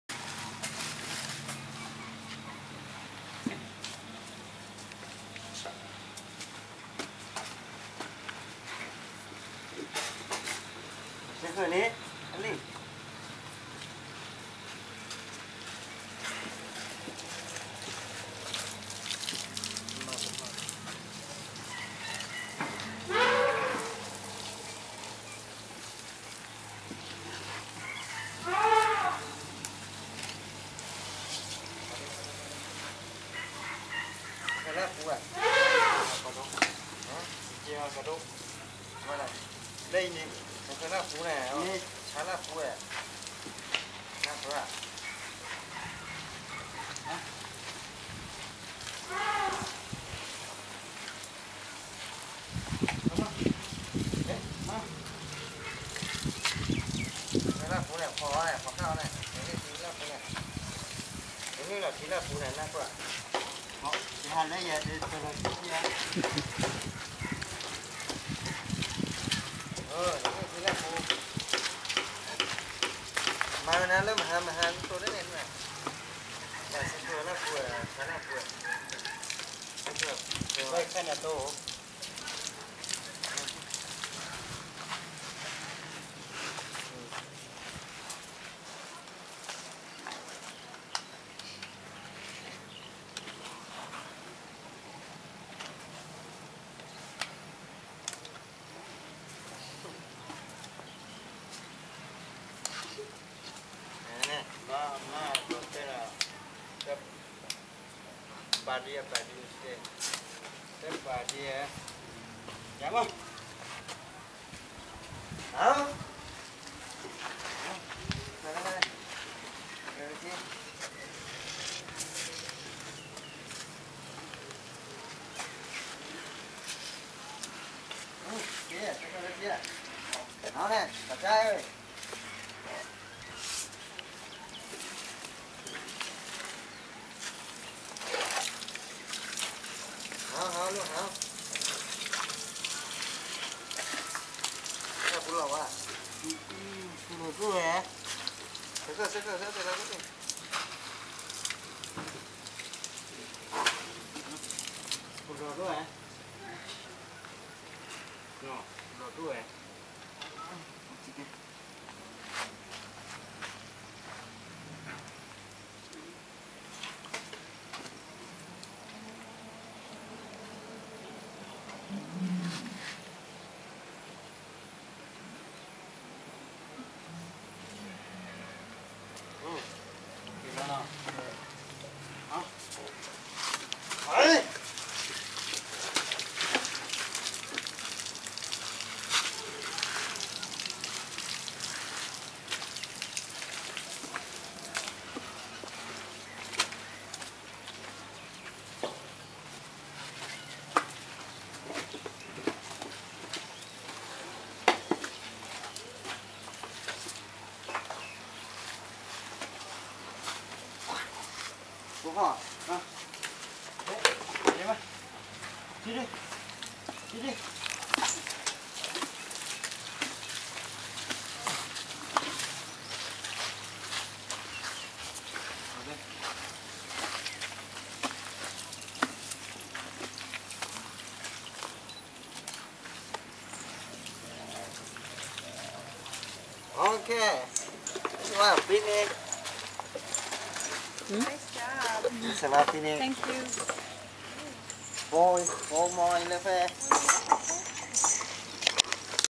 Elephant veterinarians
Not as much fun as the other videos, but it's a big part of what goes on at the park. Here, the vets tend to one of the elephants (I'm pretty sure it's Kwanjai), trying to make sure she can heal and her abscesses do not become bigger problems.